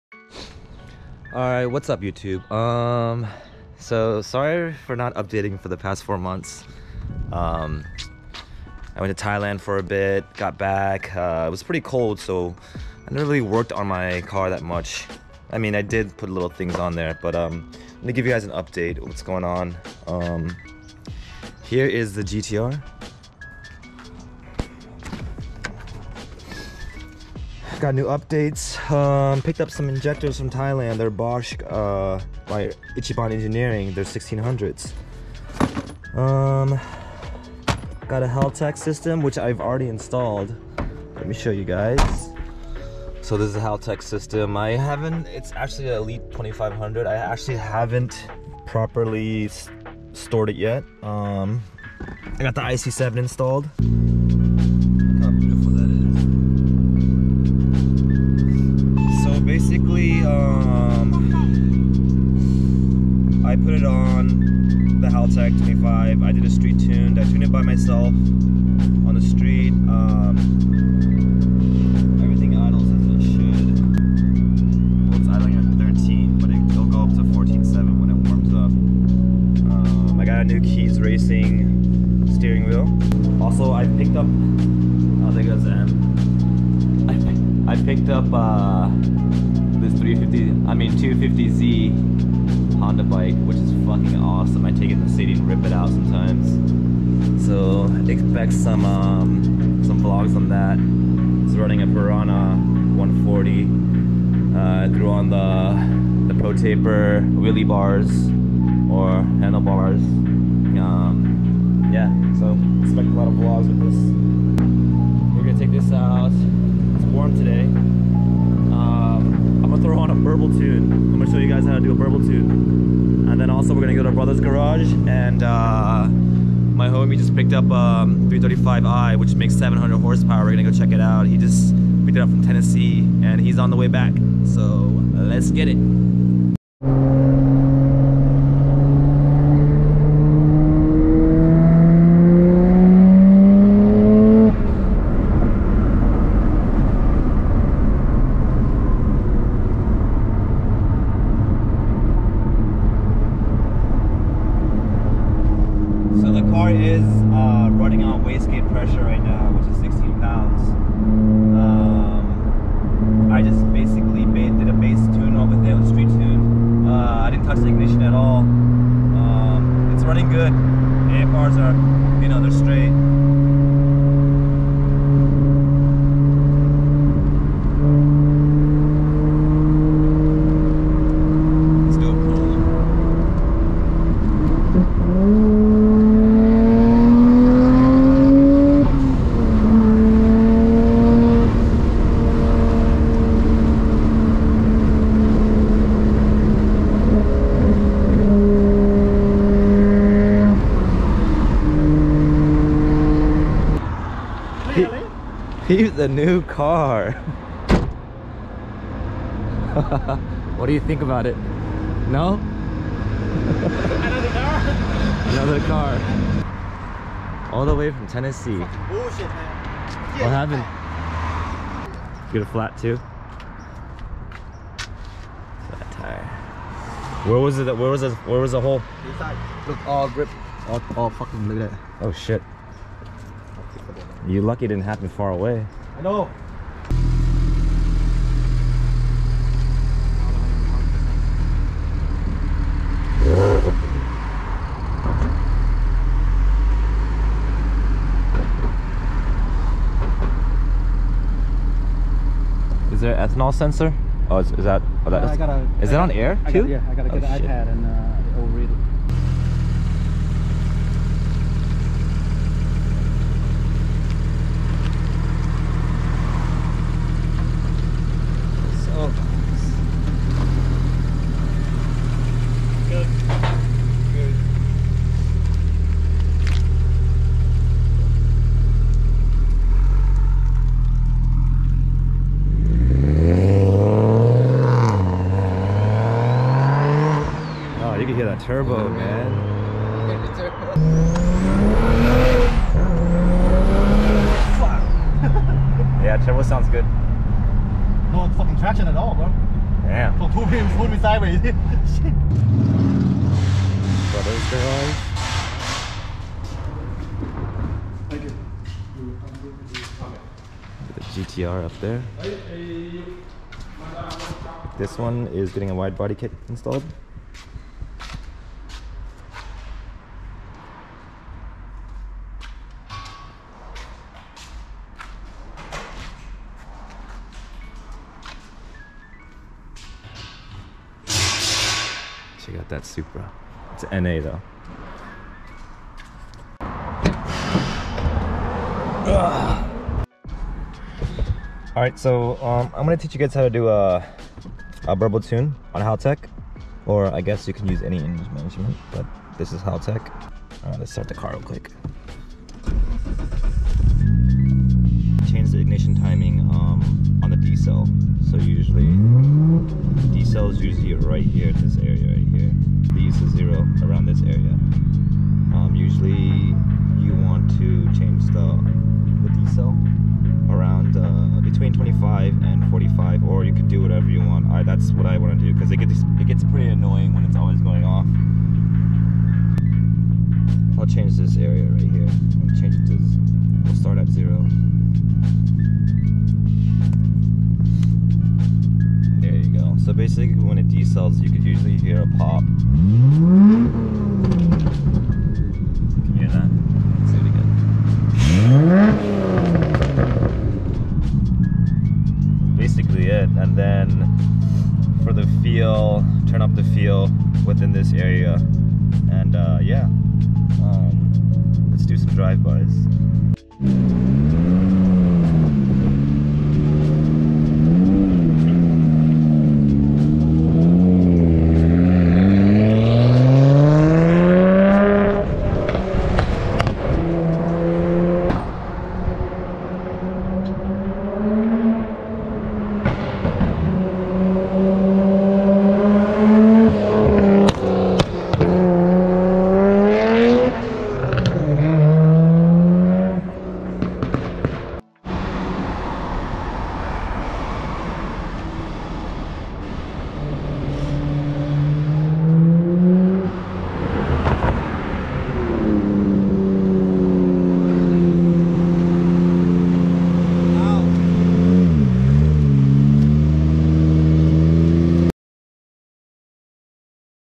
MY FRIEND BUYS A 700HP 335i - SHOTGUN TUNE ON MY R32 GTR - GTR UPDATES
Gave my R32 GTR a burble shotgun tune.  Its pretty loud so I am probably going to remove it soon.   Also a few updates to the car.  Weather is getting warmer so expect more updates.

IG Wesleyjina